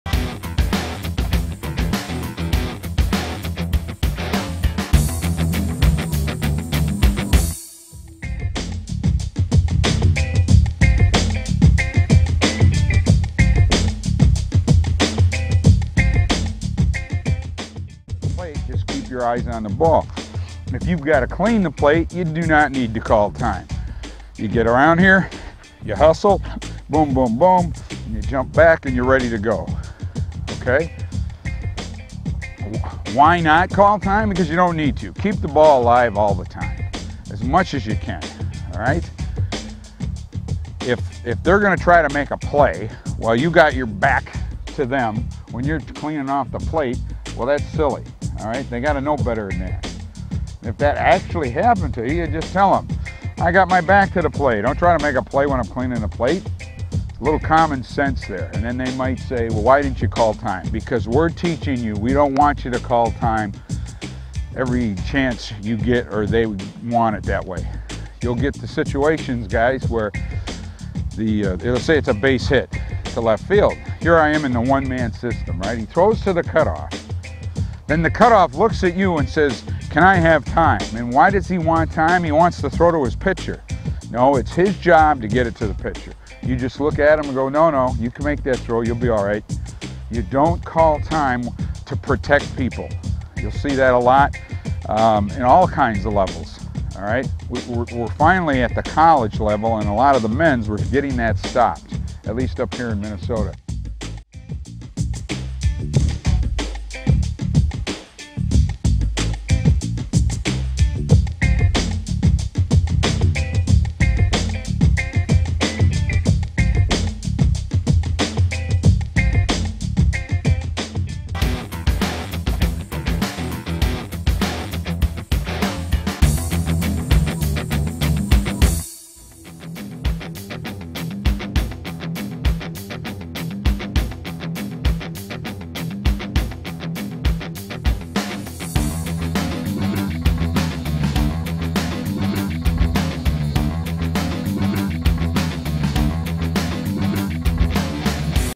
Umpire Training - Don't call time...ALL the time!
A quick umpire video lesson about not calling time every time someone wants it or when you're cleaning the plate.